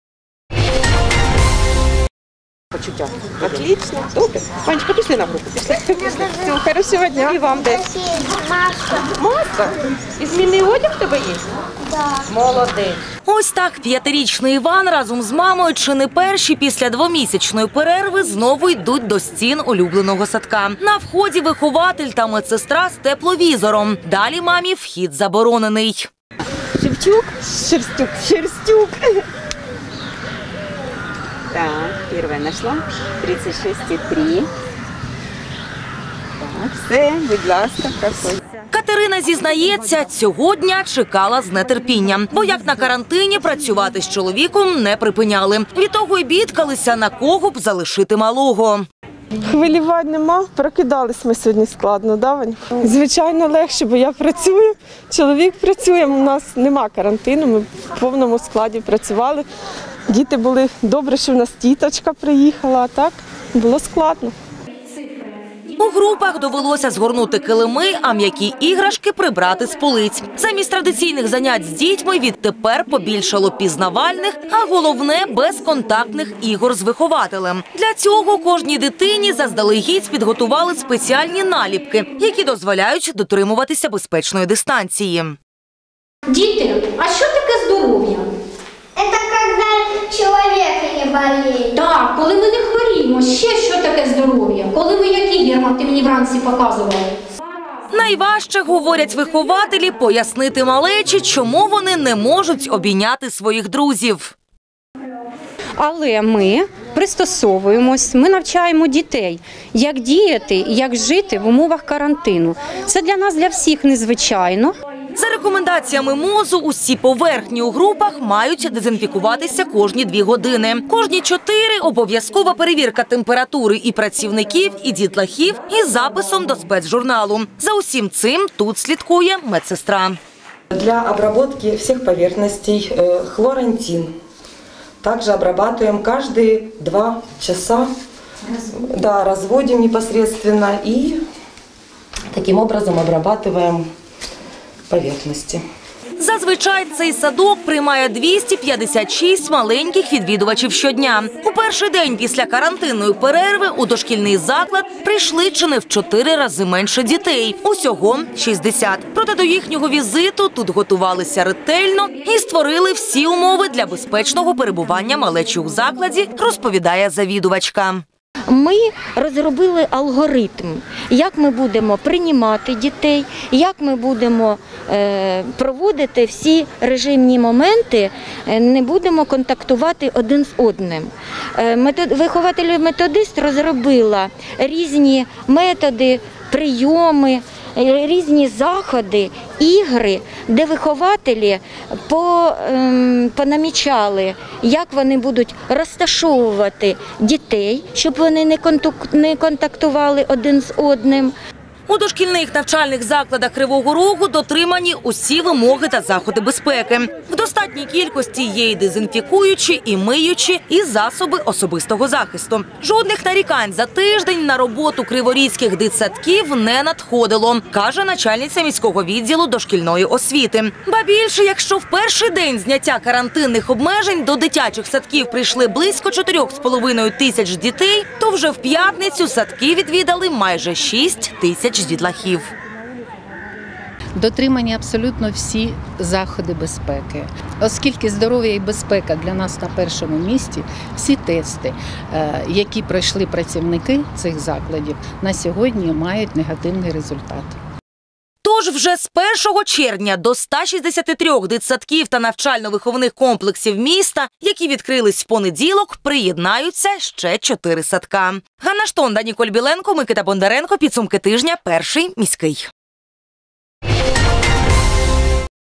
У Кривому Розі запрацювали дитячі садочки
З 25 травня дитячі садочки по всій Україні відкрили свої двері. Не став винятком і Кривий Ріг. У нашому місті запрацювали 157 закладів освіти для малечі. Але не обходиться без дотримання жорстких правил. Про старт роботи садків та заходи безпеки, прописані МОЗ, дивіться у сюжеті наших кореспондентів.